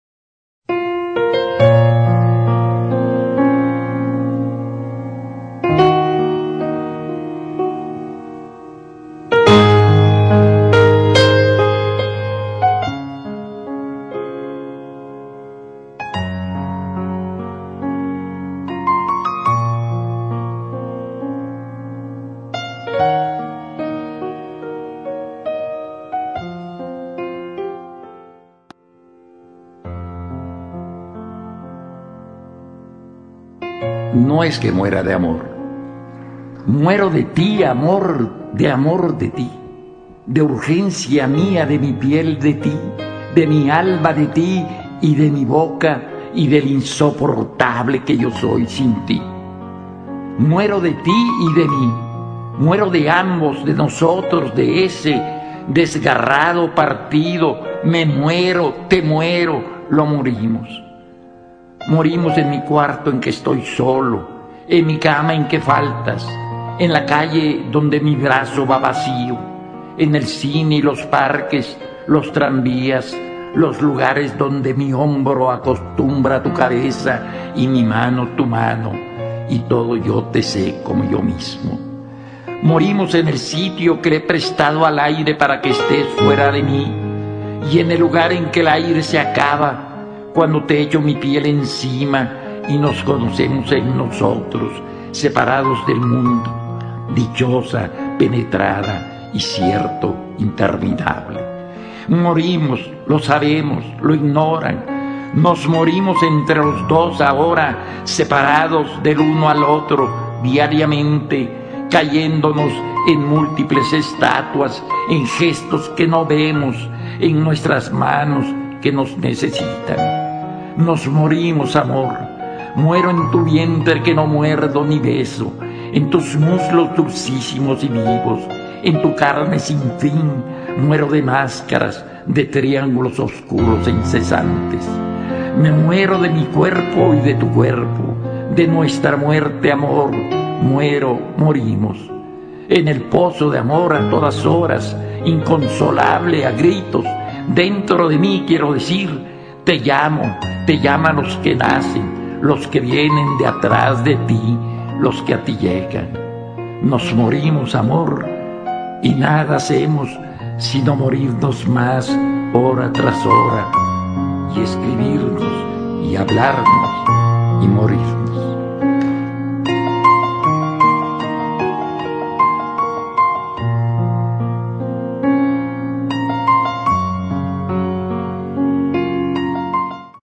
Jaime Sabines - No muero de amor, muero de ti
Poema de jaime Sabines para Imagenologia, espero lo disfruten !!!